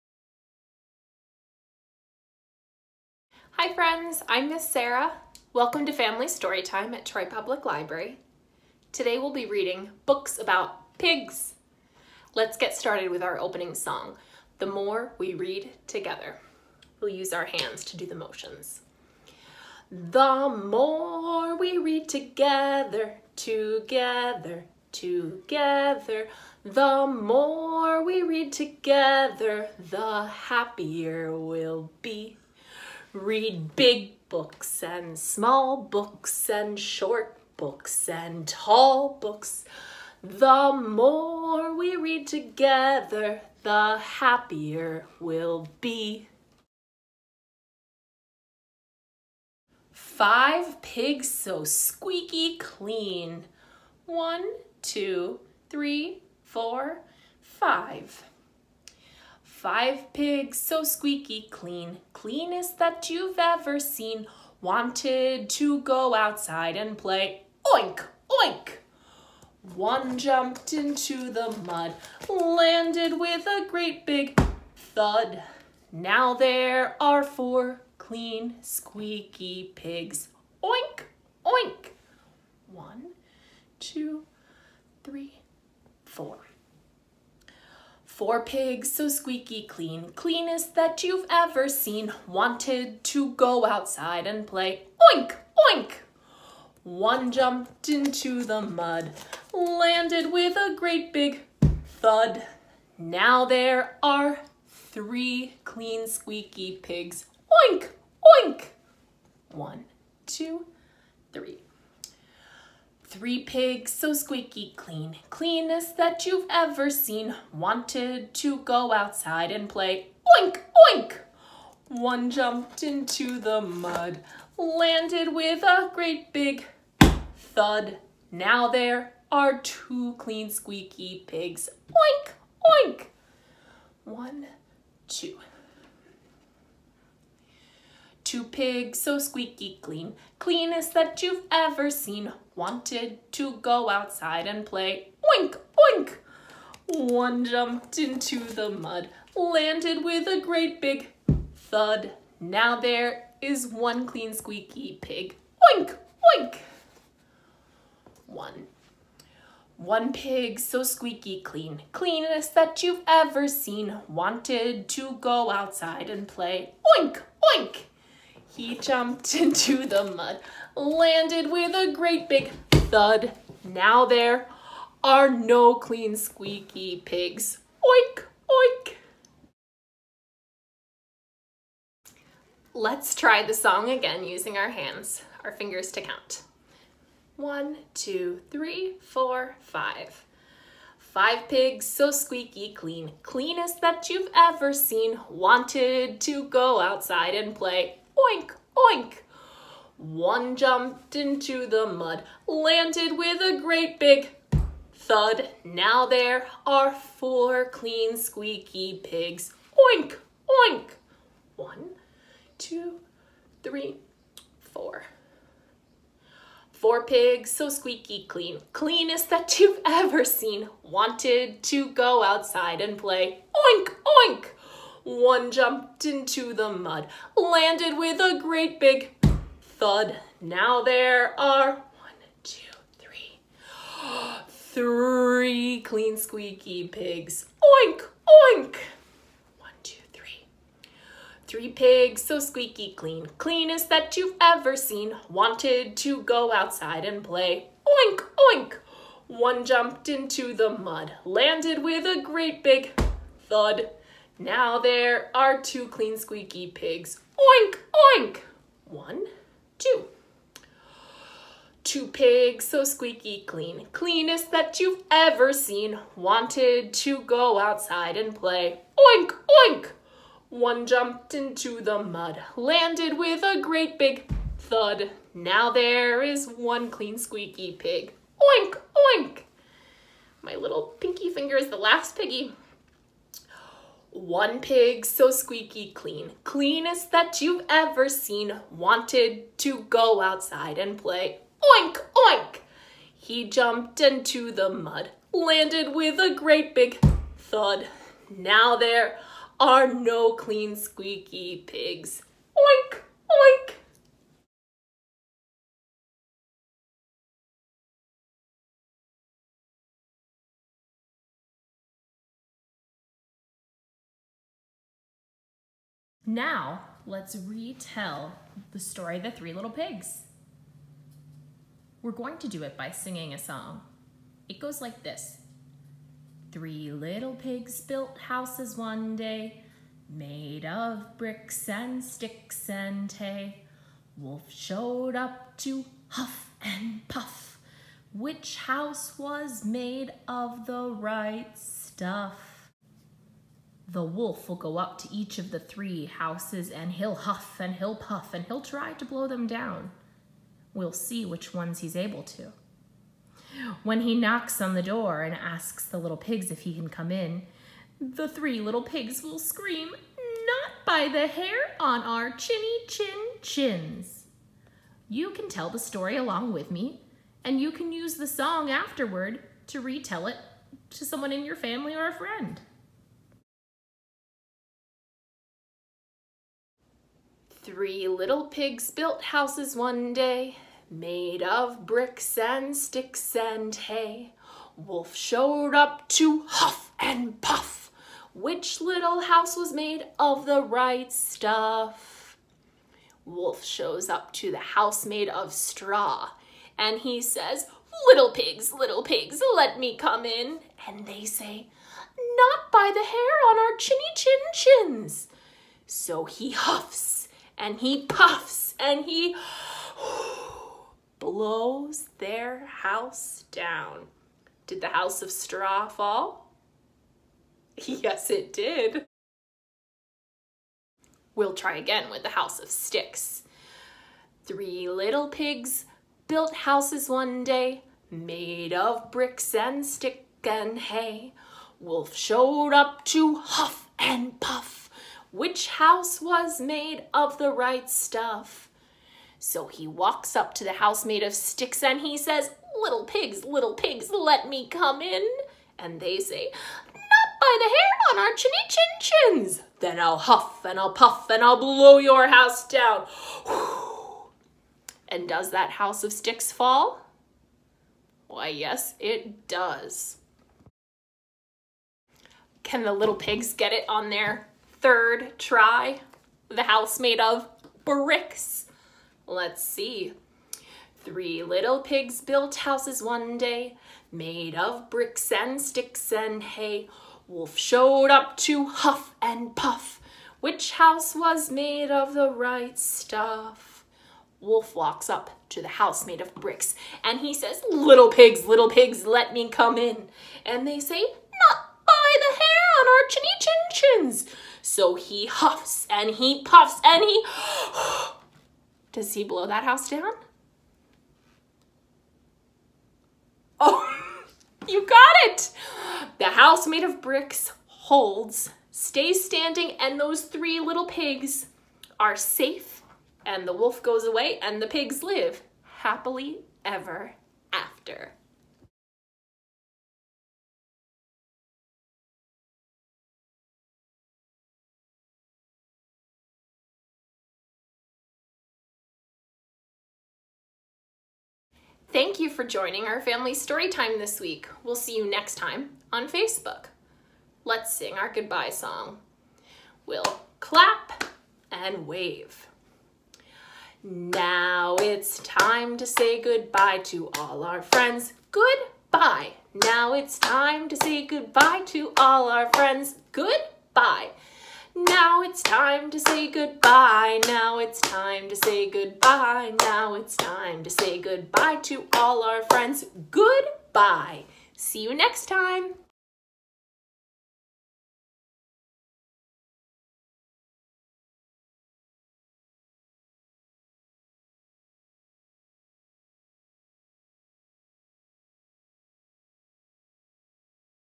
Family Storytime: Pigs
Join Ms. Sarah for songs, rhymes, and stories about pigs!